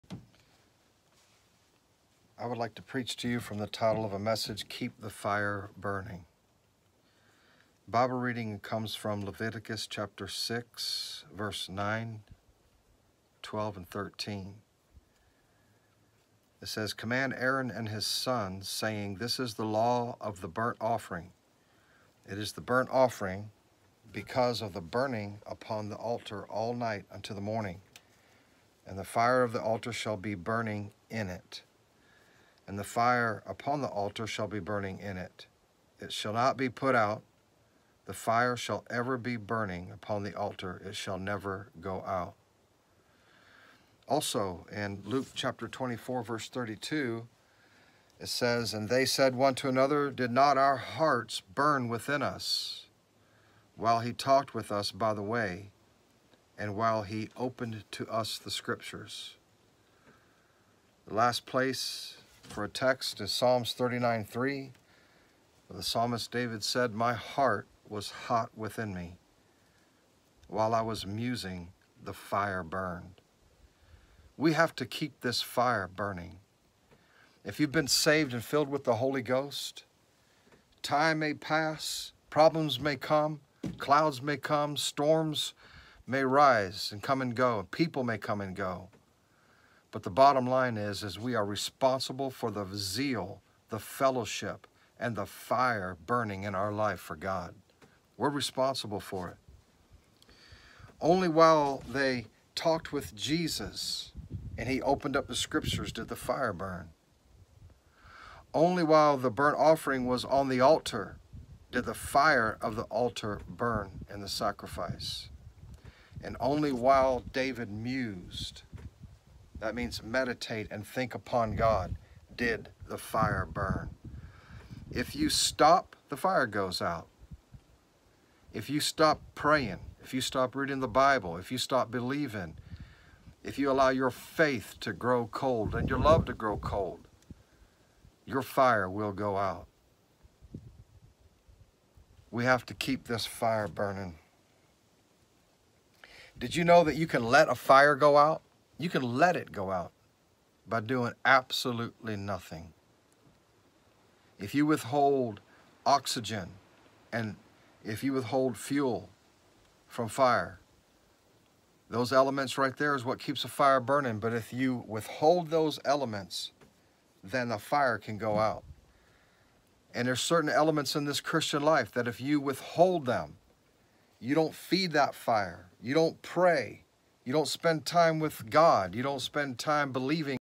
Keep the fire burning (part 1)
A message about not allowing the fire of your love, zeal, and fellowship with the Lord to go out.